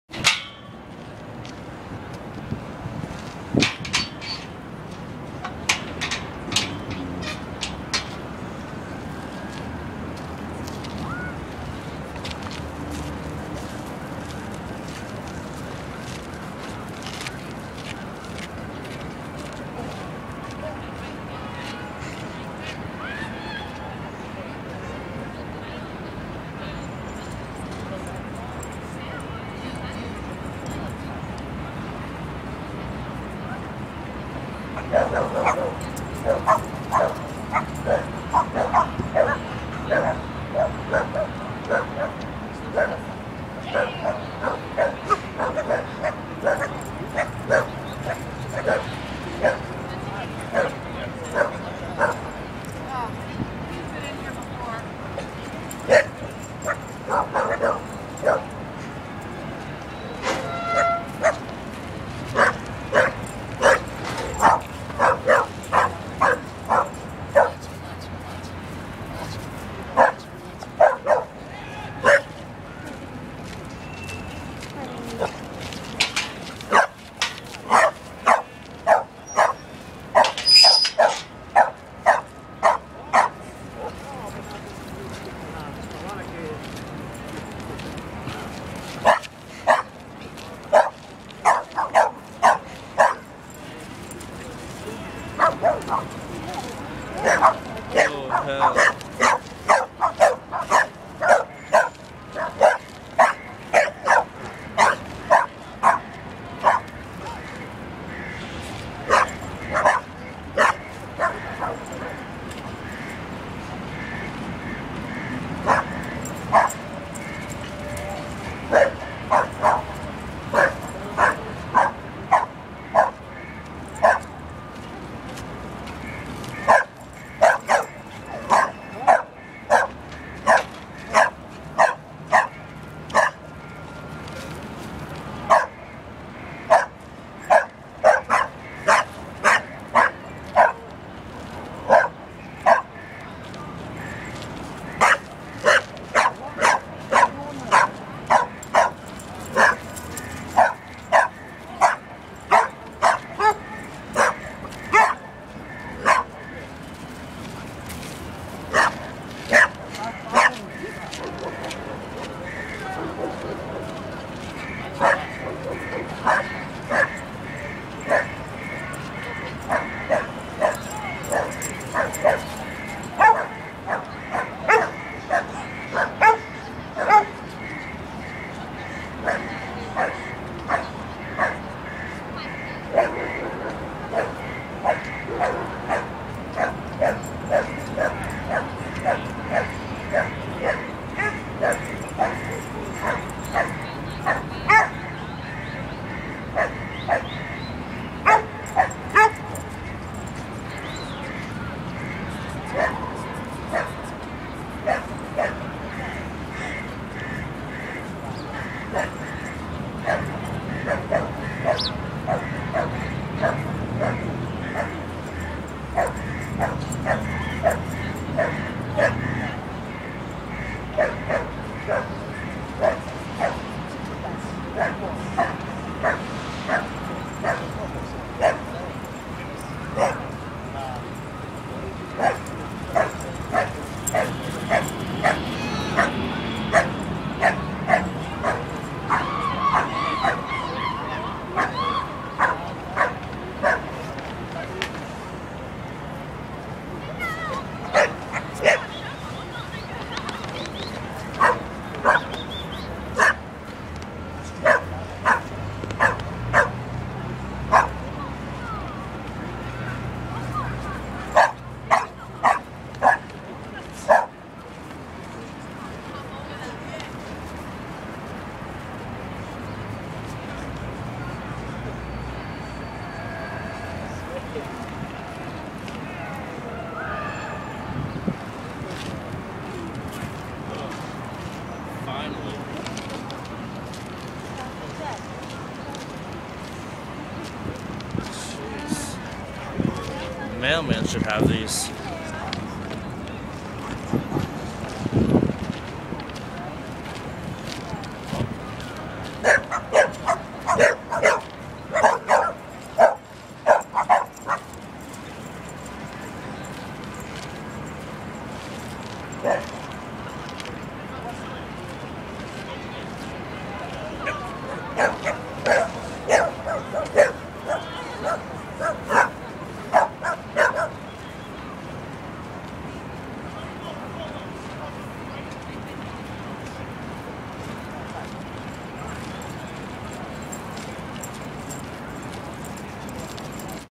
Pitbull dog attacks guy in dog park on stilts
dog attack